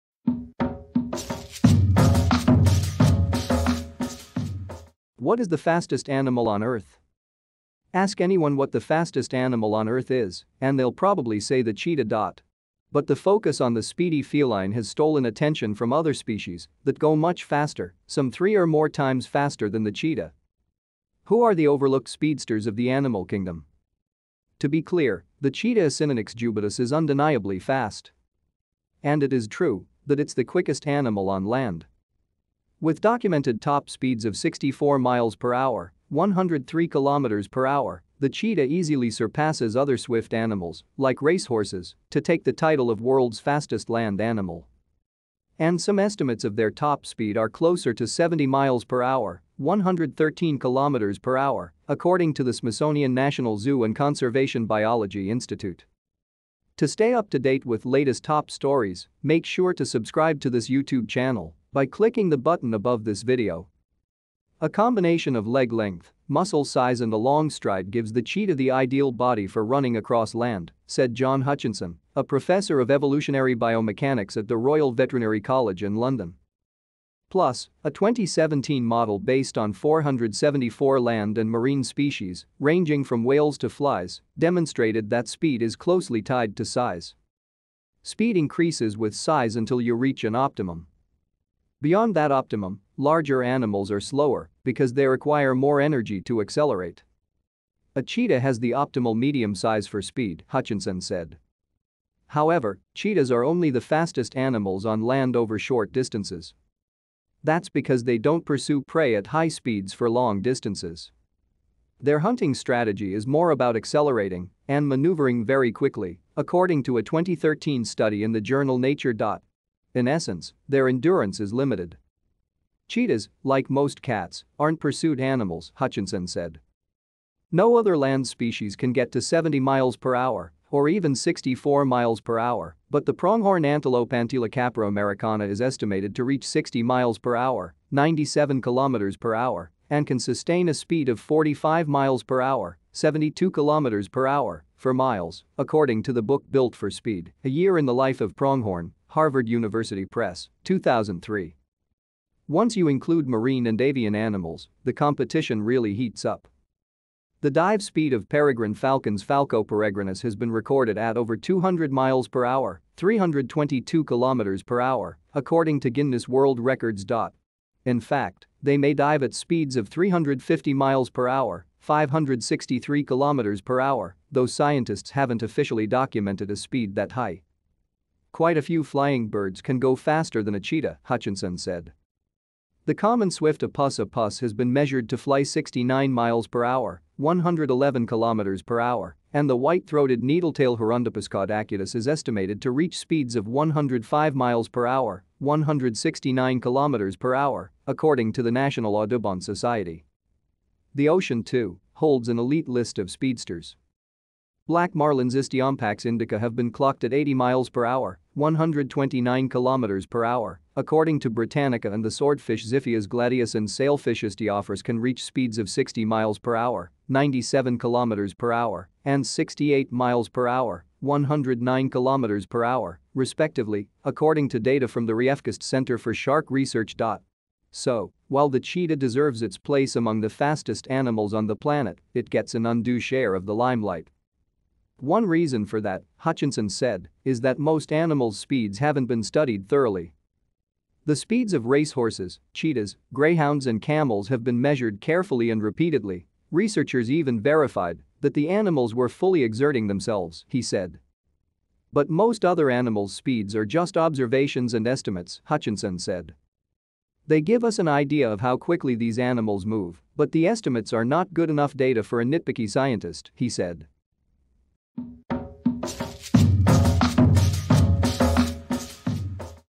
What is the fastest animal on Earth?
Ask anyone what the fastest animal on Earth is, and they'll probably say the  cheetah .
But the focus on the speedy feline has stolen attention from other species that go much faster — some three or more times faster than the cheetah.
Who are the overlooked speedsters of the animal kingdom?
To be clear, the cheetah ( Acinonyx jubatus ) is undeniably fast.
And it is true that it's the quickest animal on land.
With documented top speeds of  64 mph  (103 km/h), the cheetah easily surpasses other swift animals, like racehorses, to take the title of world's fastest land animal.
And some estimates of their top speed are closer to 70 mph (113 km/h), according to the  Smithsonian National Zoo & Conservation Biology Institute.
A combination of leg length, muscle size and a  long stride  gives the cheetah the ideal body for running across land, said John Hutchinson, a professor of evolutionary biomechanics at the Royal Veterinary College in London.